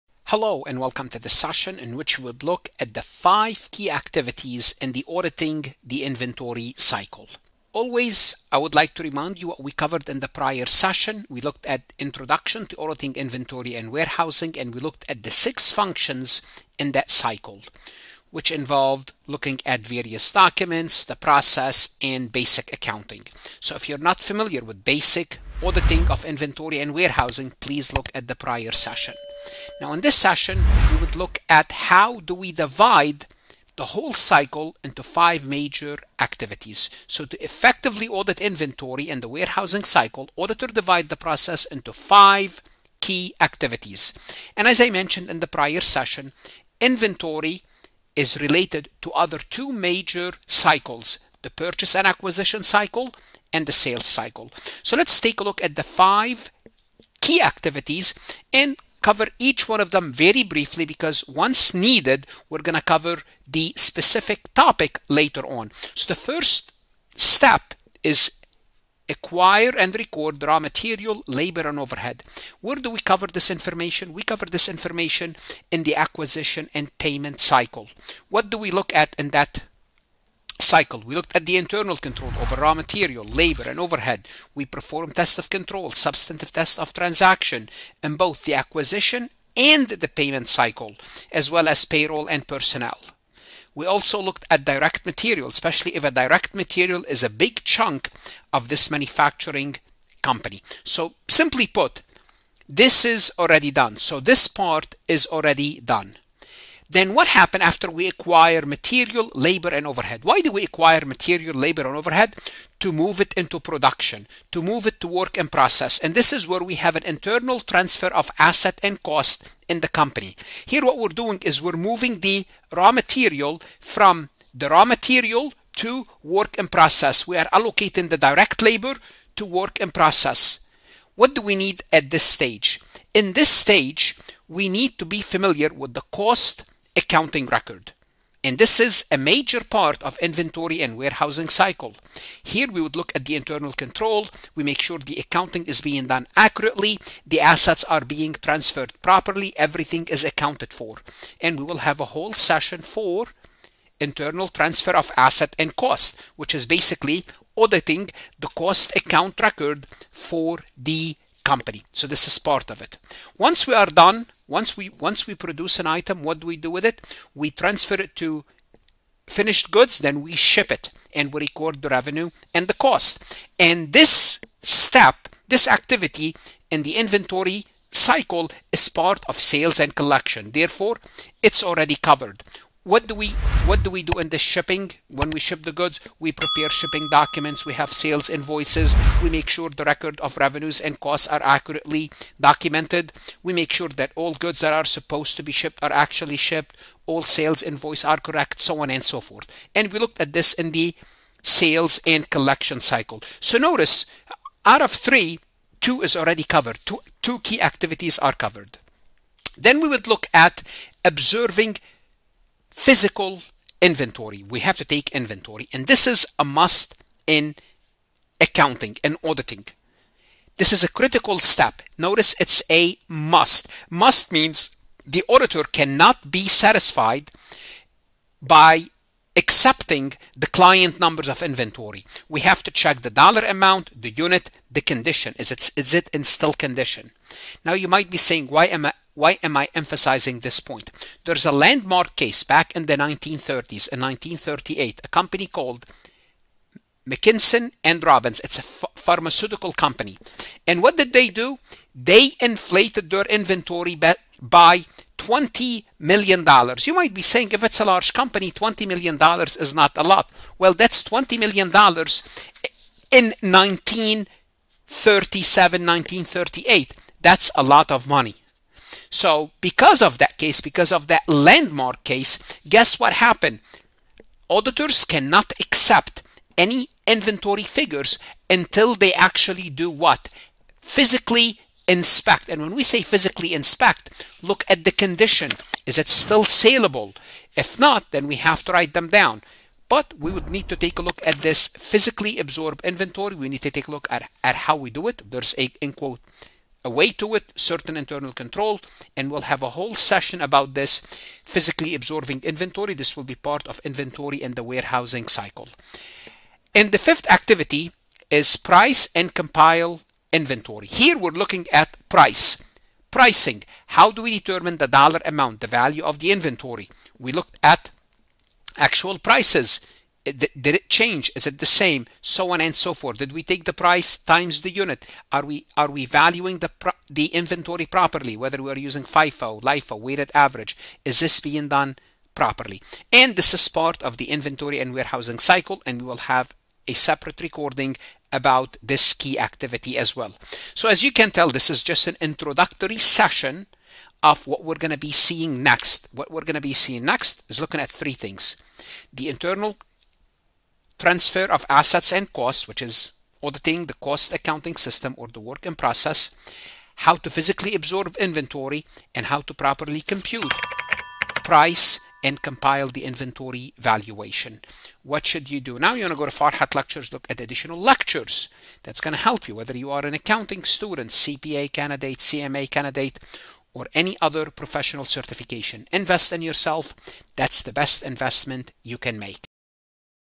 Five Parts Audit of Inventory and Warehousing Cycle | Auditing Course
This lecture by Professor Farhat provides a comprehensive explanation of the five audit of the inventory and warehousing cycle.

0:00 Introduction
Acquire and record raw material, labor, and overhead (1:07): This involves looking at internal controls and testing transactions within the acquisition and payment cycle.
Internal transfer of assets and costs (1:47): This step focuses on moving raw materials to work in process and allocating direct labor, requiring familiarity with cost accounting records.
Shipping and recording revenue/costs (2:50): This activity is part of the sales and collection cycle, ensuring accurate documentation of revenues and costs.
Observing physical inventory (3:31): A critical step where auditors physically inspect inventory to verify its condition and quantity.
Price and compile inventory (5:08): This involves determining the dollar value of the inventory and ensuring proper valuation methods are used.

The audit of the inventory and warehousing cycle can be divided into five activities within the cycle:

Acquire and record raw materials, labor, and overhead
Internally transfer assets and costs
Ship goods and record revenue and costs
Physically observe inventory
Price and compile inventory
The auditor obtains an understanding of internal controls over these three functions and then performs tests of controls and substantive tests of transactions in both the acquisition and payment cycle and the payroll and personnel cycle. These tests should satisfy auditors that controls affecting the acquisitions of raw materials and manufacturing costs are operating effectively and that acquisition transactions are correctly stated. When direct labor is a significant part of manufactured inventory, auditors should verify the proper accounting for these costs in the payroll and personnel cycle.

Transfer Assets and Costs
Clients account for these activities in the cost accounting records, which are independent of other cycles and are tested as part of the audit of the inventory and warehousing cycle.
Physically Observe Inventory

Auditors are required by auditing standards to observe the client taking a physical inventory count to determine whether recorded inventory actually exists at the balance sheet date and is correctly counted by the client. Physical examination is an essential type of evidence used to verify the existence and count of inventory.

Costs used to value inventory must be tested to determine whether the client has correctly followed an inventory method that is both in accordance with accounting standards and consistent with previous years. Audit procedures used to verify these costs are called price tests. In addition, the auditor must perform inventory compilation tests, which are tests to verify whether the physical counts were correctly summarized, the inventory quantities and prices were correctly extended, and the extended inventory correctly footed to equal the general ledger inventory balance.
The physical observation of inventory and the pricing and compilation of the inventory are audited using substantive analytical procedures and tests of details of balance.